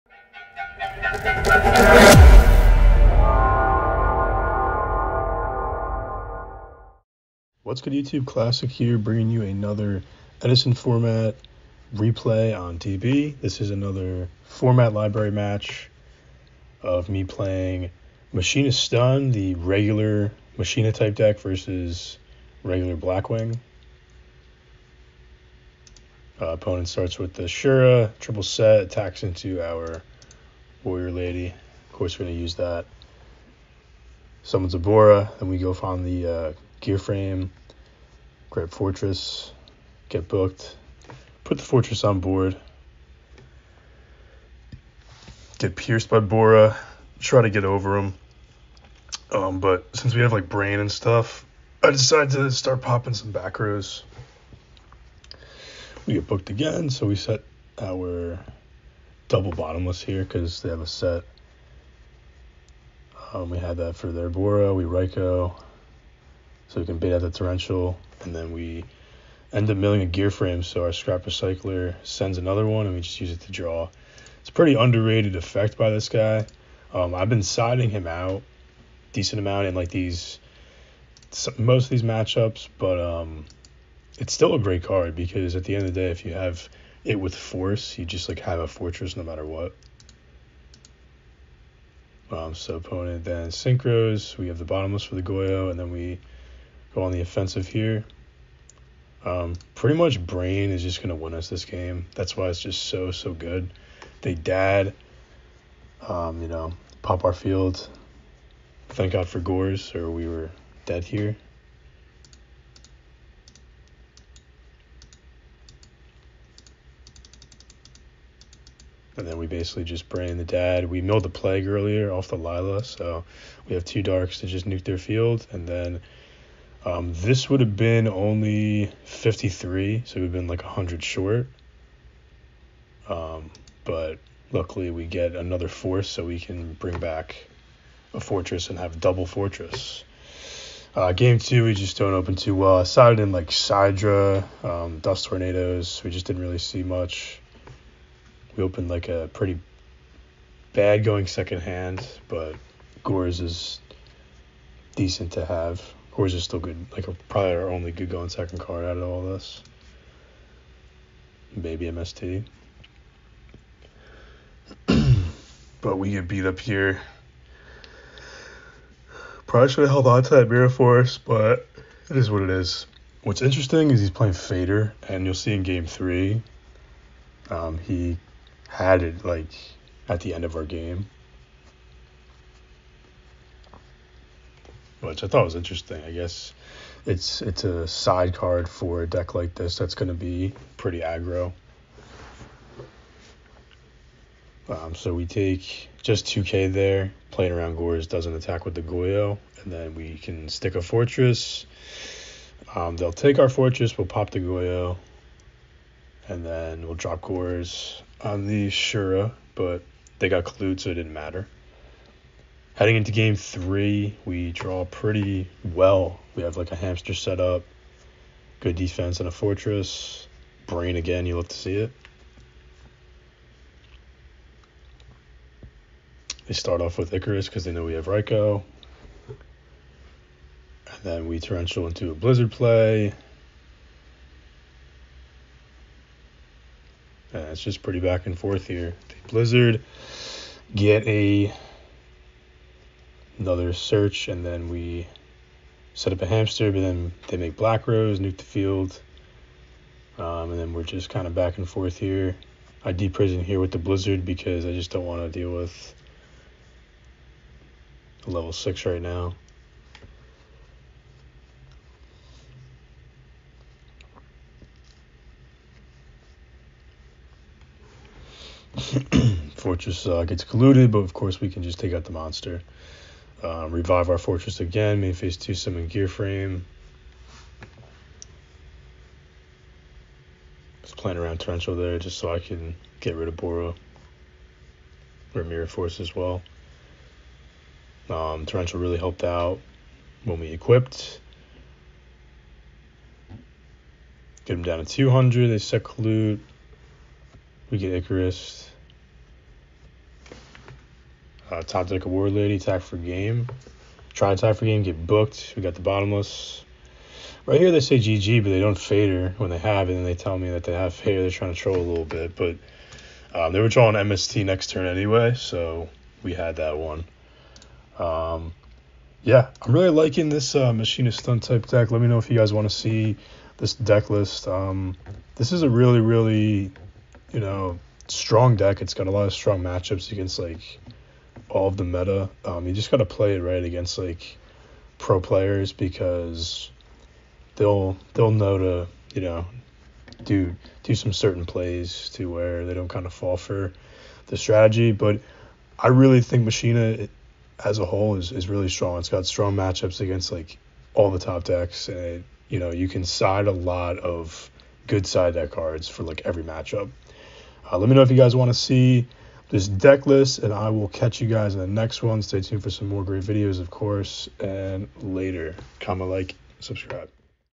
Machina Stun VS Blackwings (Edison Format)
Machina Stun (me) vs. Blackwings (undefxned) replay. :D When this video hits 10 likes, I'll do a machina deck profile!